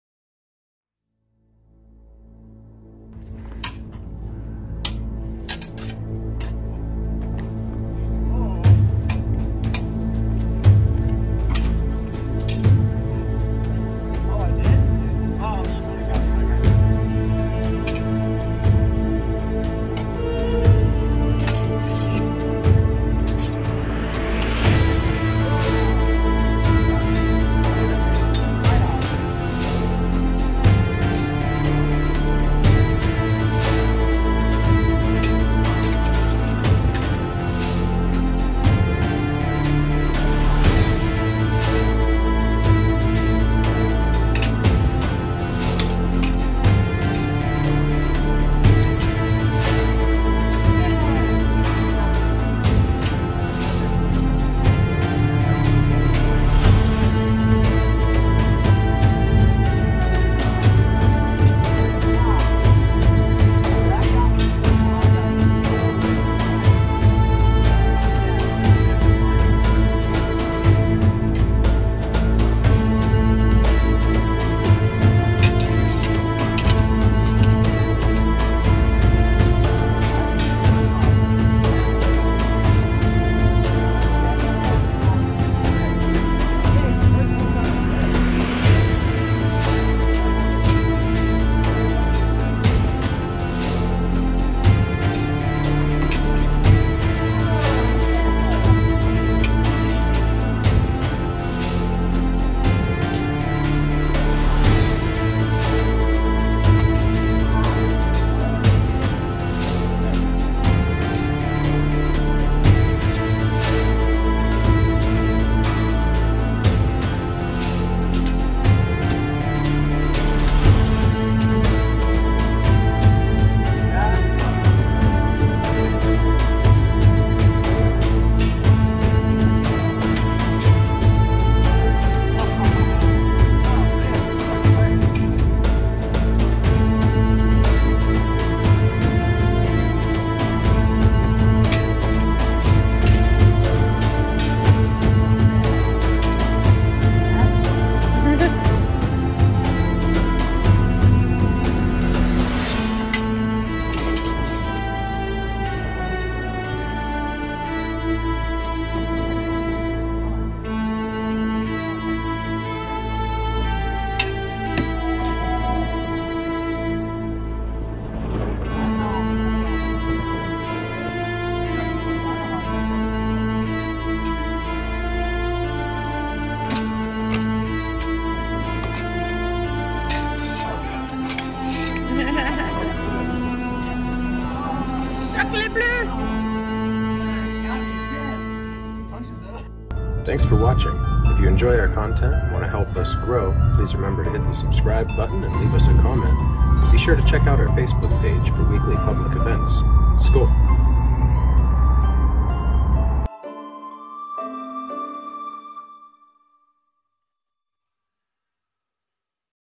Sword/Buckler vs Longsword Mixed HEMA Sparring
Kyle (Kilt) vs Malachi sparring with the longsword against the broadsword and buckler (Or rather, padding we're substituting as a buckler for now)